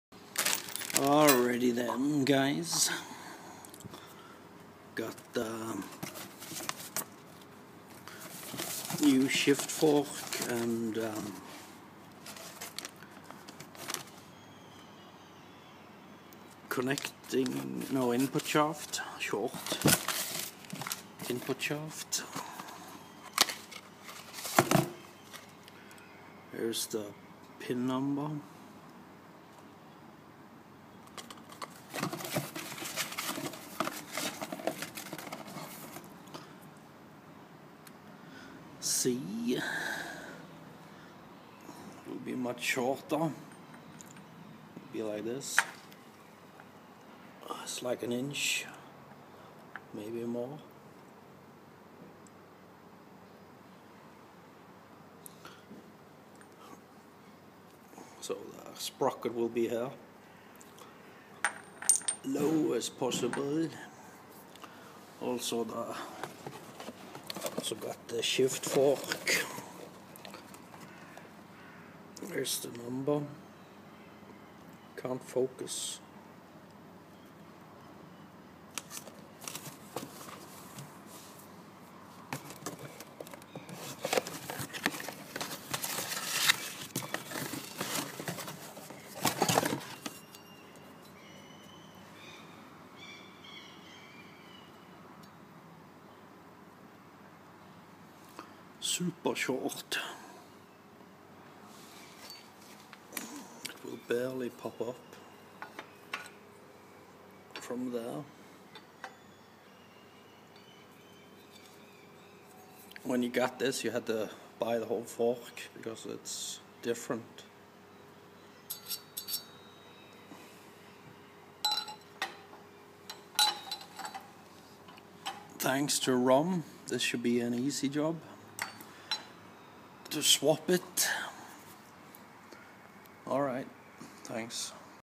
Peerless 820 -037 5-Speed Gearbox, short Input shaft and sh
Peerless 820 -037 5-Speed Gearbox, short Input shaft and shifter rod & fork.

Thanks too ROM this will be an easy swap.